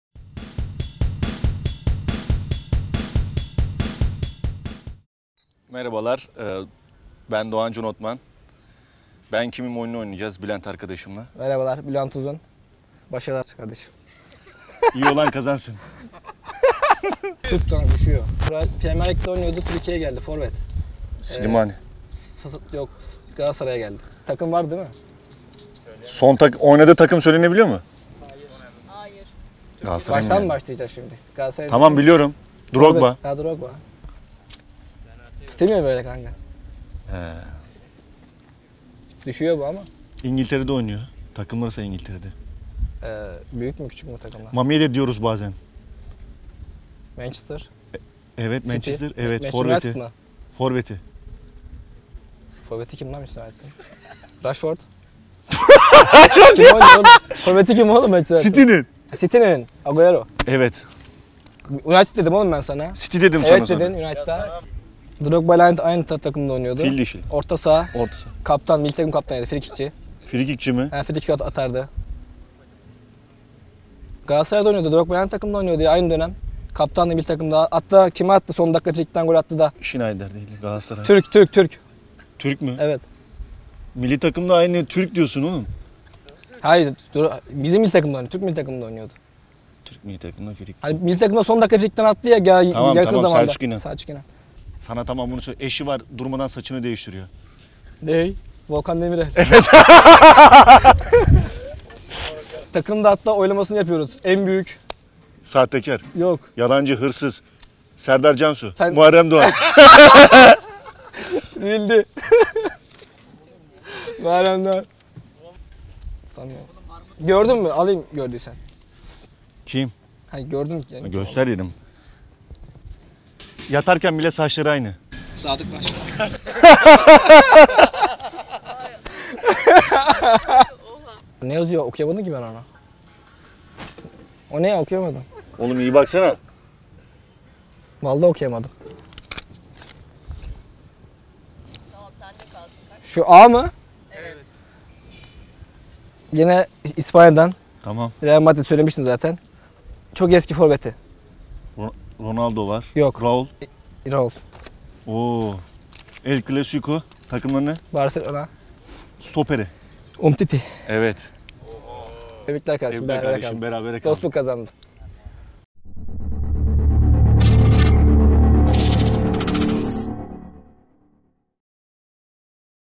Bülent Uzun ve Doğan Can Otman İle Ben Kimim?
Futbolcularımız Bülent Uzun ve Doğan Can Otman ile keyifli bir oyun oynadık. 🥳

Bakalım birbirlerinin alnında yazan isimleri tahmin edebilecekler mi? 👊🏻